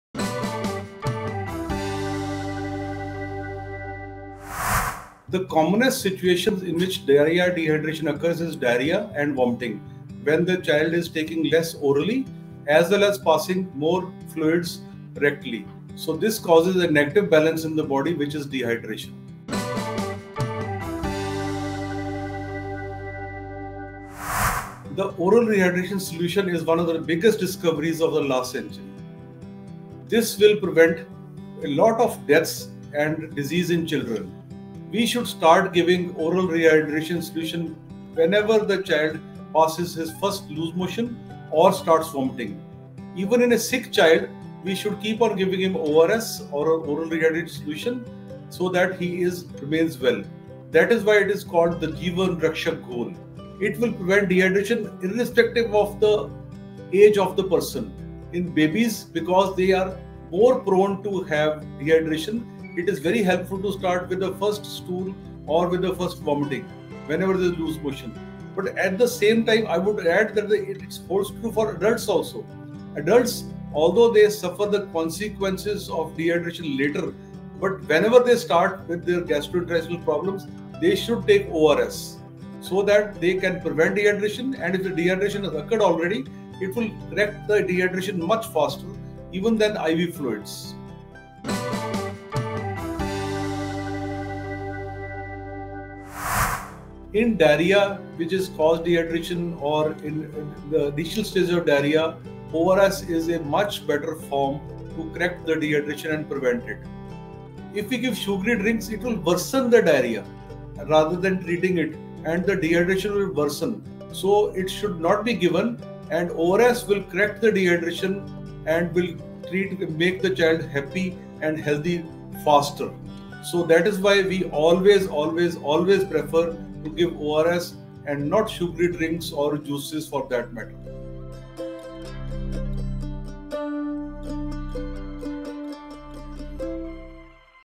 HydrationForHealth By Electral | ORS & Diarrhoeal Dehydration by Dr Sarvinder Singh
Diarrheal dehydration can lead to severe health issues. Oral Rehydration Solution contains specific levels of sodium, potassium, and glucose to help prevent dehydration faster than other sugary drinks.

Listen to Dr. Sarvinder Singh, M.D.(Paediatrics), D.C.H., F.I.C.M.C.H., Consultant Child Specialist & Neonatologist, talking about management of dehydration in children with Oral Rehydration Solution over other beverages.